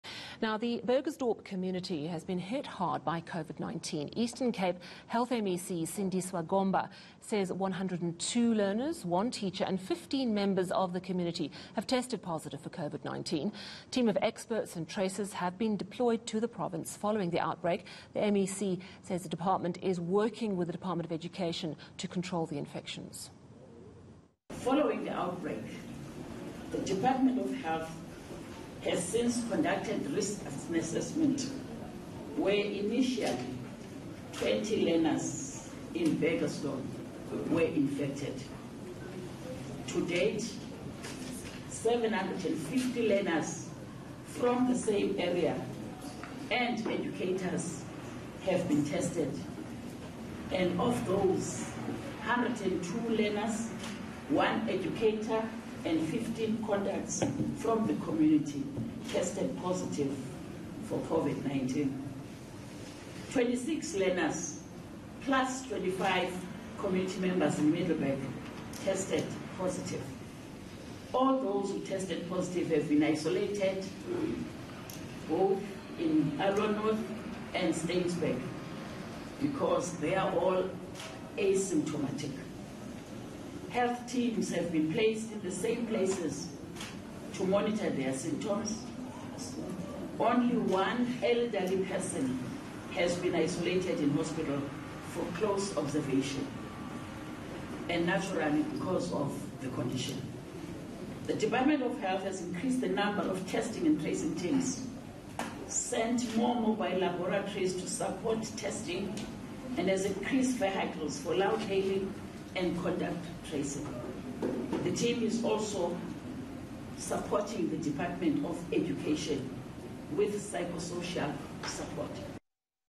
Outbreak of COVID-19 in the Eastern Cape
Eastern Cape Health MEC Sindiswa Gomba says 102 learners, one teacher and 15 members of the Burgersdorp community have tested positive for COVID-19. Courtesy of DStv403